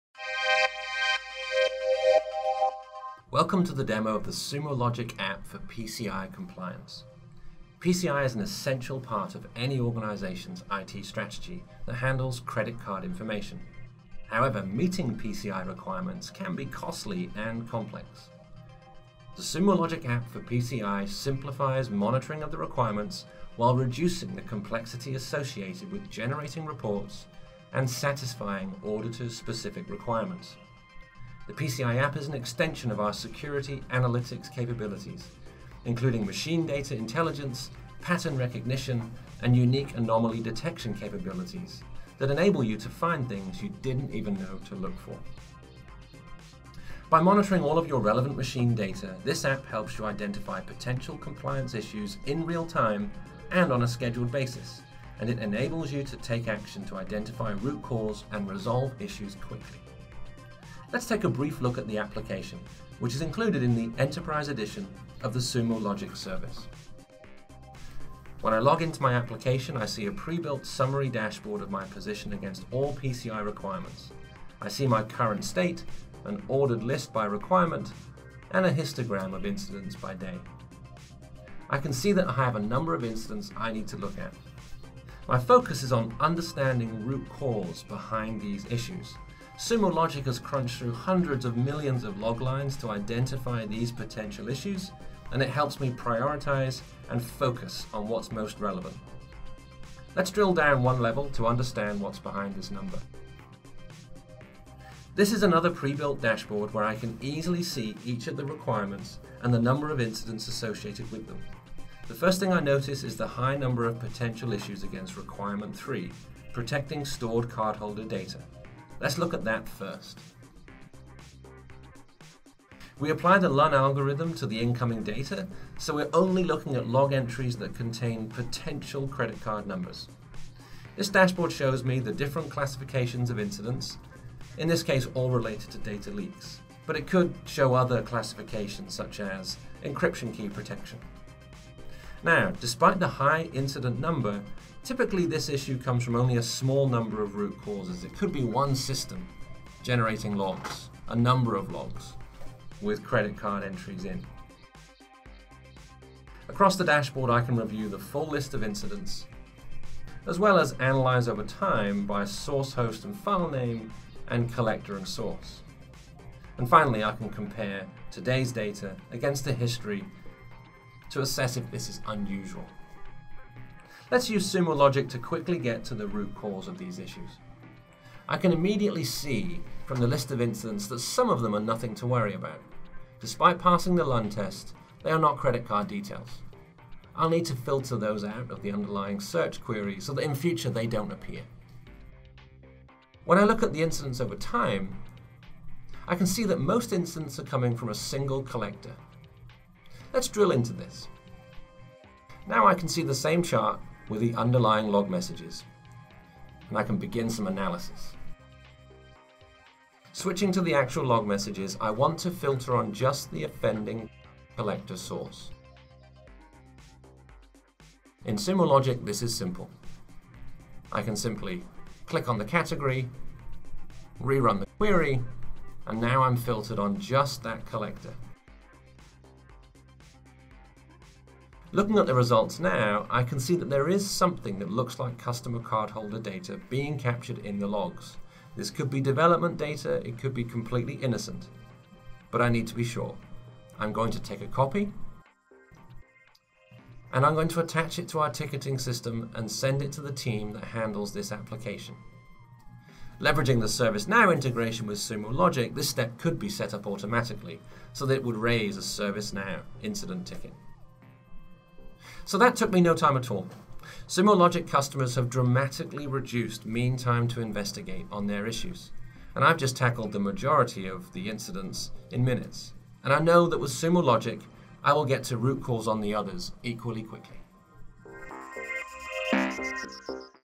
Sumo Logic Application for PCI Compliance
This video will give a brief overview of the Sumo Logic Application for PCI Compliance. The Sumo Logic application for PCI compliance is a natural addition to the company's Enterprise Security Analytics solutions and helps organizations of any size meet the stringent PCI Data Security Standards.